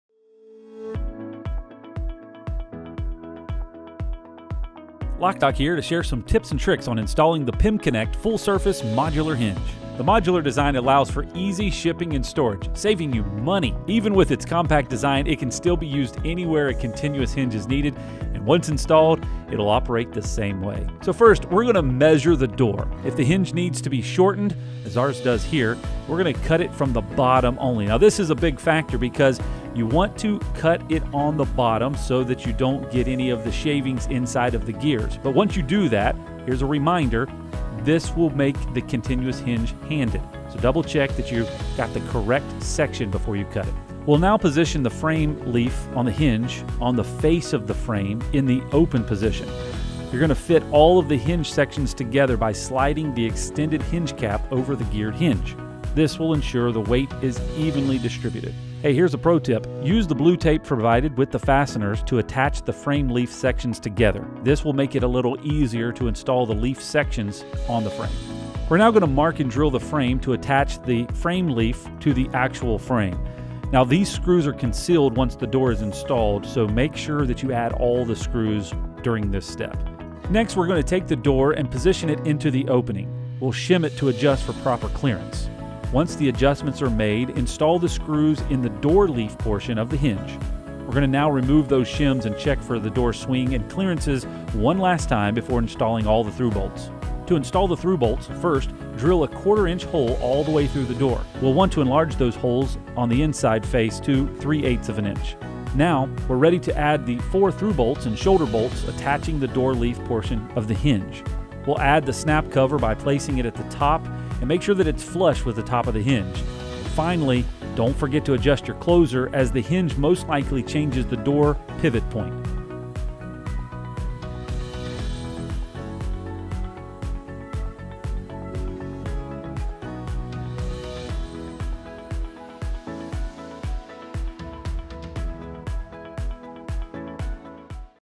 PemKonnect™ Full Surface Modular Continuous Hinge Installation Video
Learn how to install Pemko's Full Surface Modular Continuous Hinge. Keep an eye out for pro-tips, helping to make installation smooth and simple.